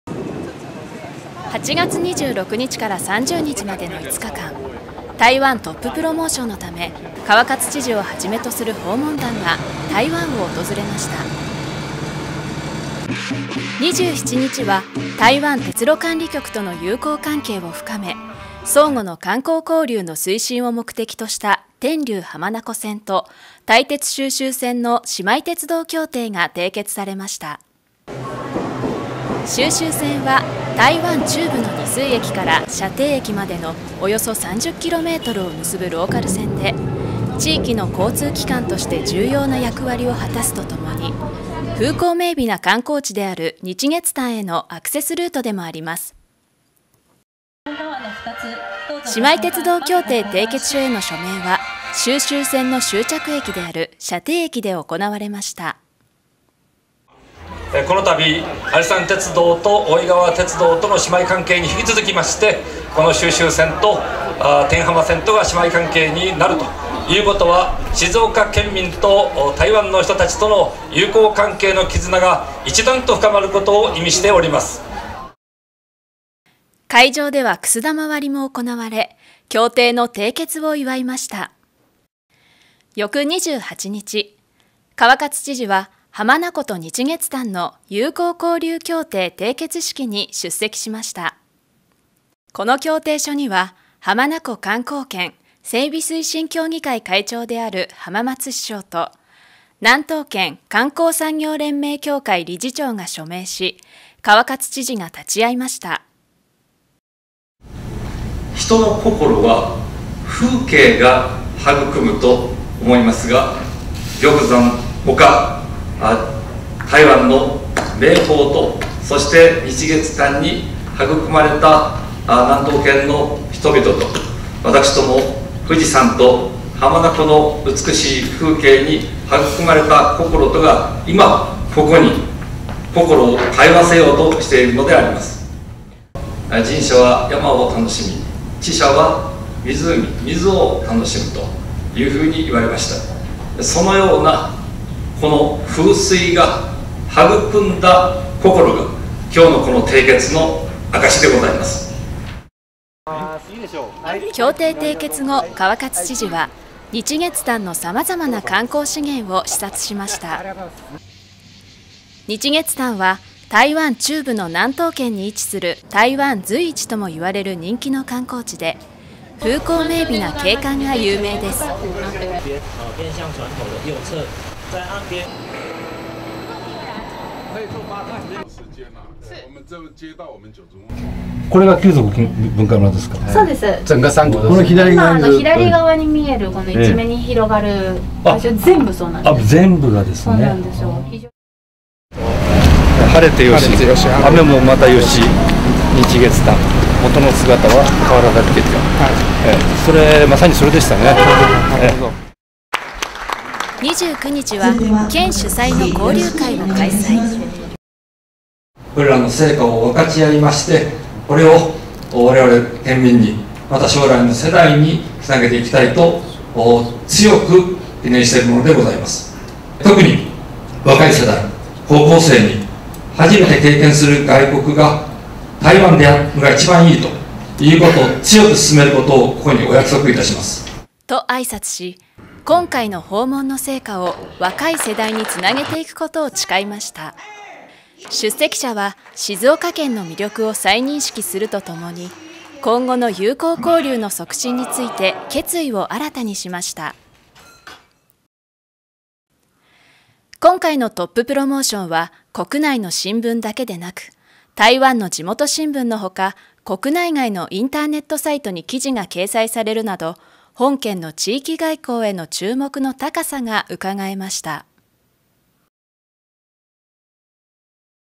台湾訪問_160826-0829
8月26日から30日までの５日間、台湾トッププロモーションのため、川勝知事をはじめとする訪問団が、台湾を訪れました。